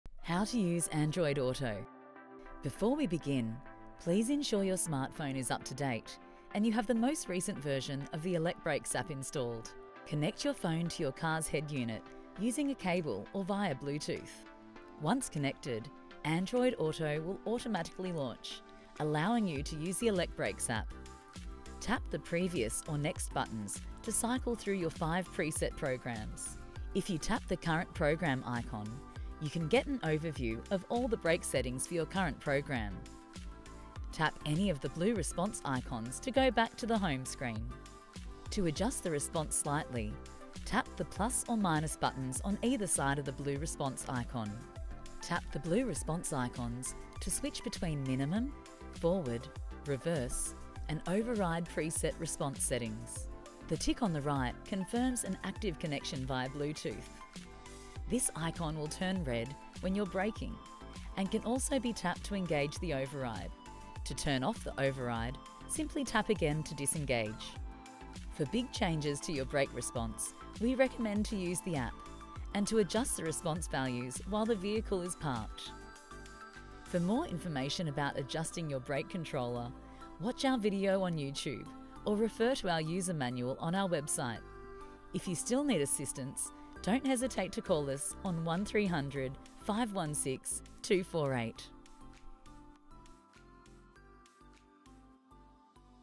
How to use Android Auto with Elecbrakes
Welcome to our Elecbrakes tutorial! In this video, we'll guide you through the simple steps to set up Andriod Auto with your Elecbrakes brake controller. Watch as we walk you through the process, ensuring a seamless and efficient connection for enhanced control and safety.

Whether you're a first-time user or looking to optimise your setup, this video provides all the information you need to get the most out of your Elecbrakes brake controller. Enjoy the convenience of managing your brake controller directly from your Android Auto interface.

for more information.